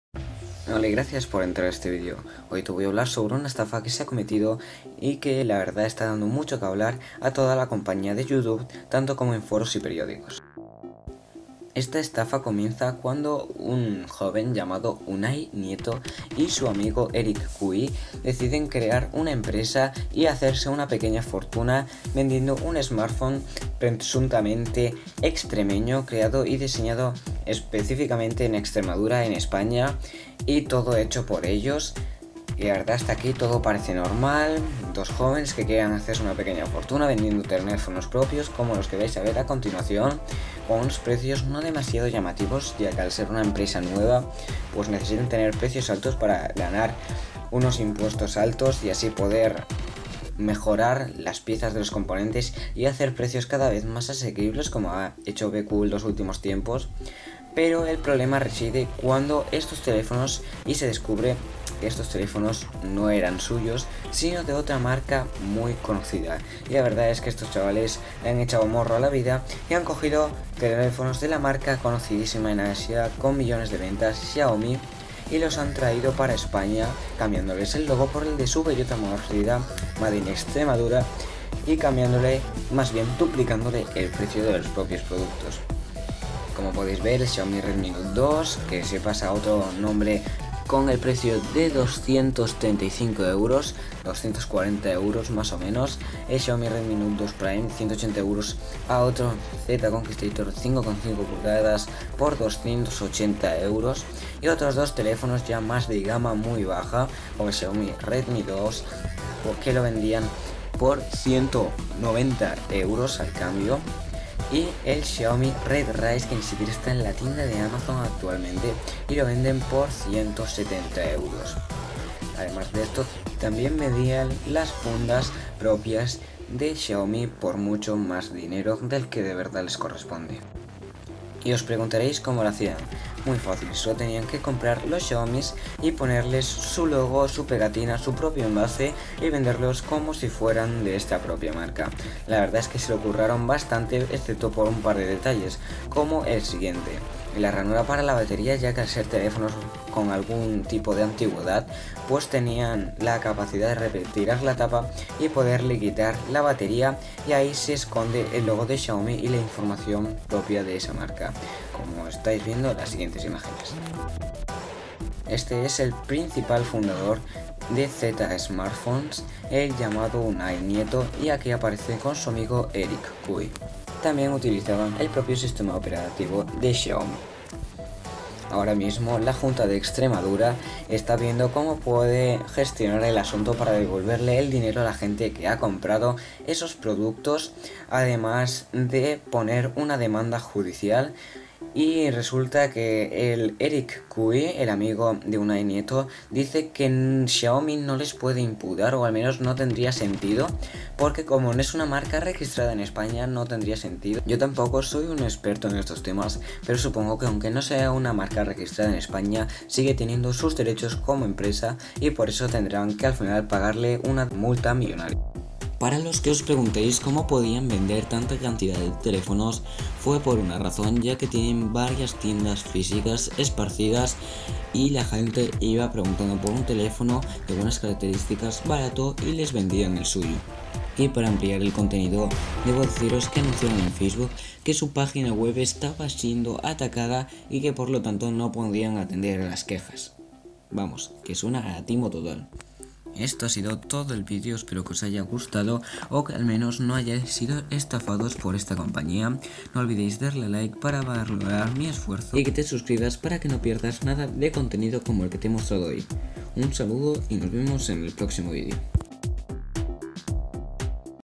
Zetta Smartphones | Toda la información
En este vídeo te enseño detalladamente la estafa cometida por una pequeña empresa que hacía pasar sus teléfonos por "Made in Extremadura"